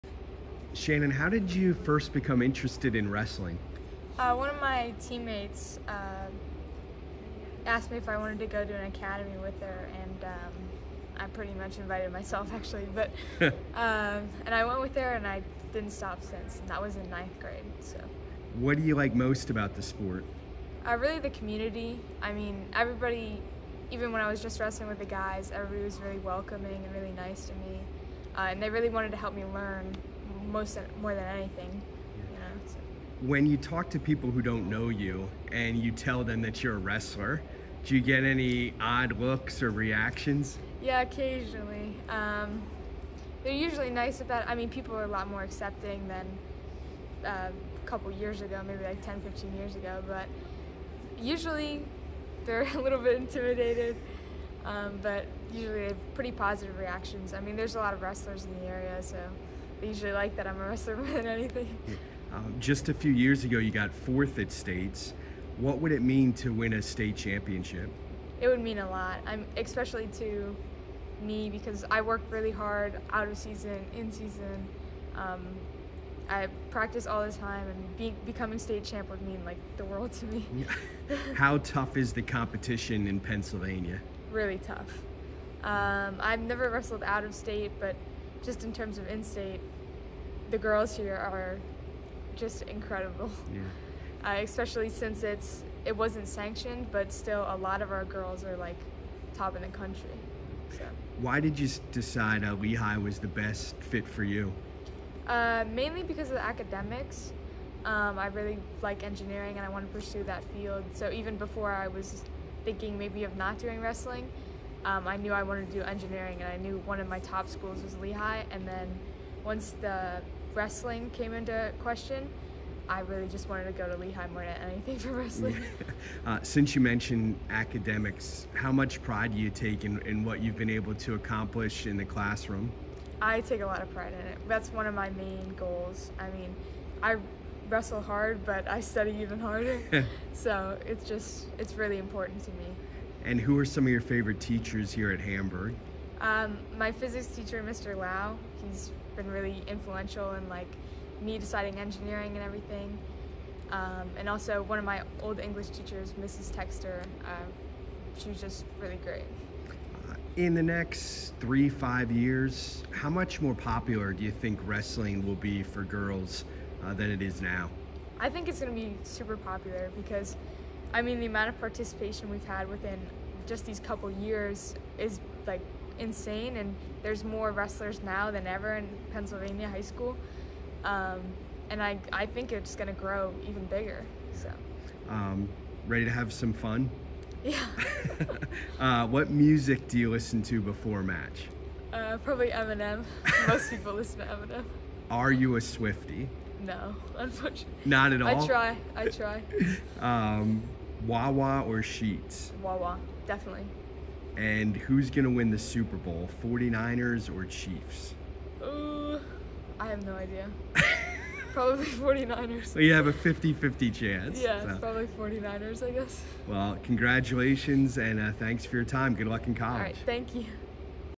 Hamburg wrestler and Lehigh commit Shannon Govern
Shannon Govern is the definition of a student-athlete.  The Hamburg senior is ranked second in the state at 130 pounds.  She's also ranked second in her class and is committed to Lehigh University to major in Electrical Engineering.  Govern is extremely focused in the classroom.  "I take a lot of pride in it. That's one of my main goals. I wrestle hard, but I study even harder. It's really important to me," she told SV Sports' Paul Roberts.  Govern has had plenty of success on the mat as well.  She finished fourth in the state two years ago at 118 pounds.  Govern won the Berks County championship this year at 130 pounds and has a record of (20-1).  The wrestling community has been very supportive.  "Everybody was really welcoming and really nice to me. They really wanted to help me learn," she replied.  Govern believes the future is bright for girls wrestling at the high school level.  "I think it's gonna be super popular. The amount of participation we've had within just a couple years is insane. There's more wrestlers now than ever in Pennsylvania," she noted.  Before heading to Lehigh, Govern is hoping to bring home a state gold medal.  "I work really hard. I practice all the time...becoming state champ would mean the world to me," she said.